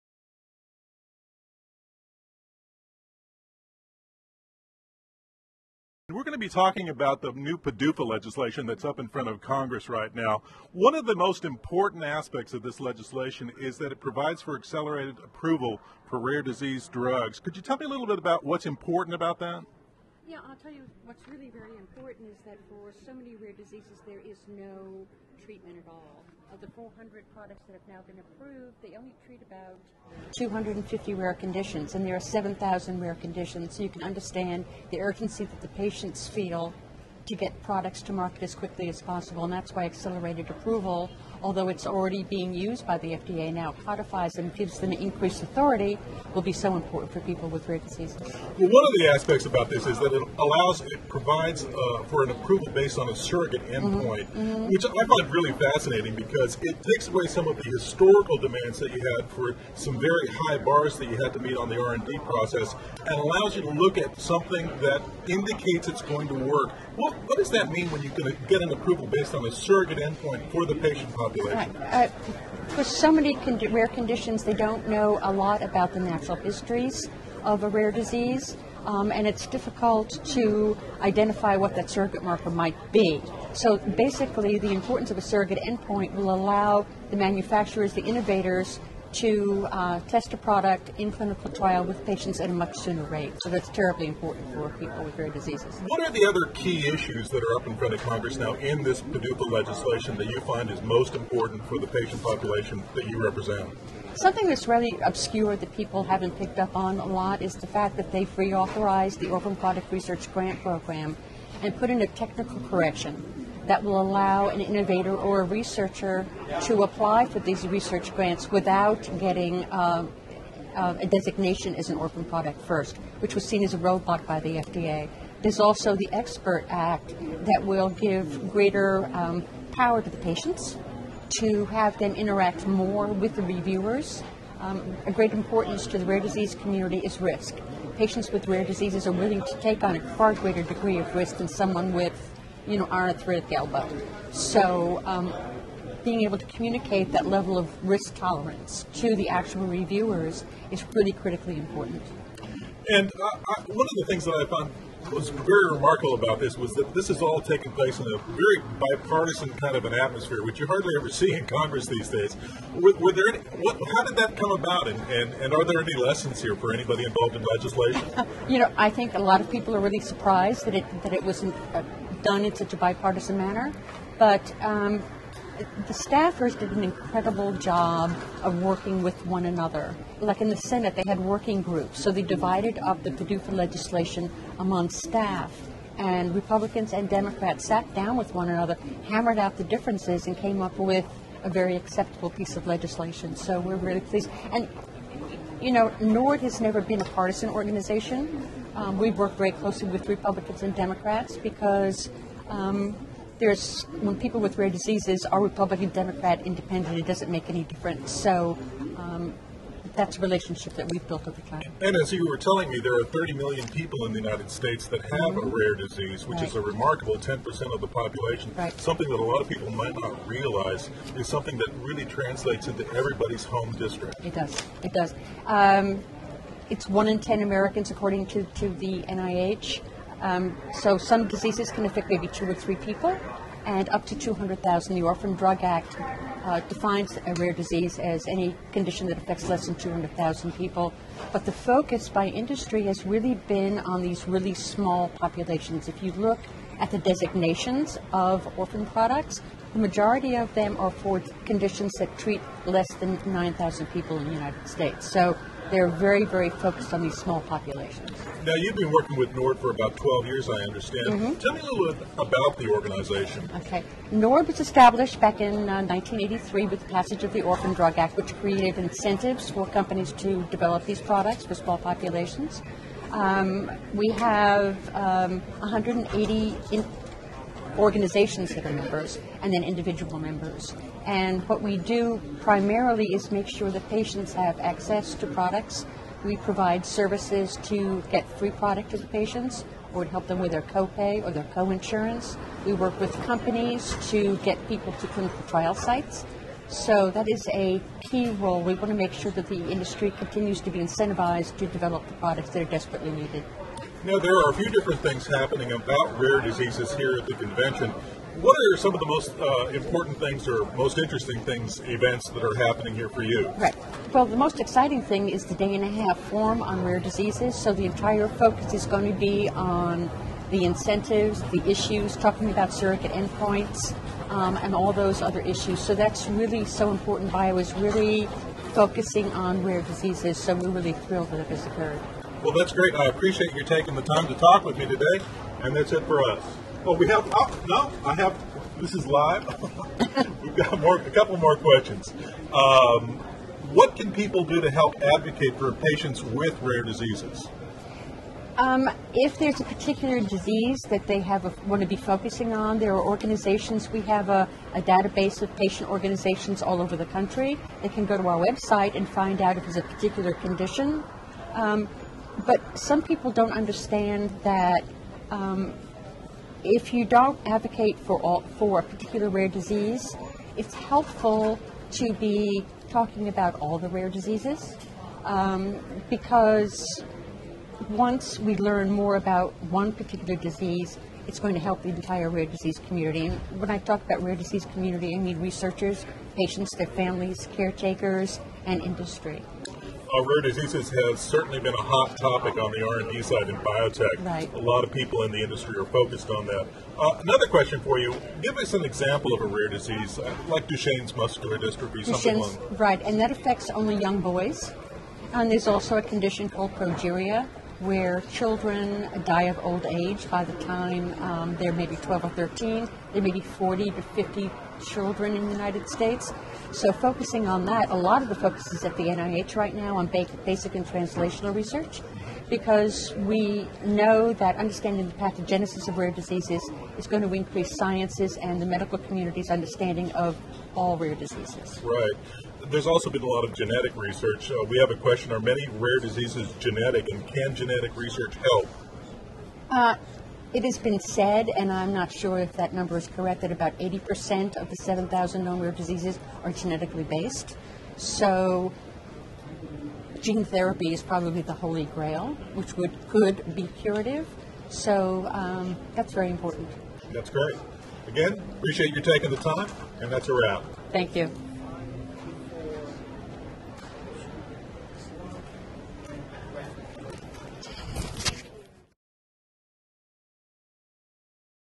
John Carroll of FierceBiotech Interviews Diane Dorman, NORD
John Carroll of FierceBiotech interviews Diane Dorman, of the National Organization for Rare Disorders, about the Prescription Drug User Fee Act.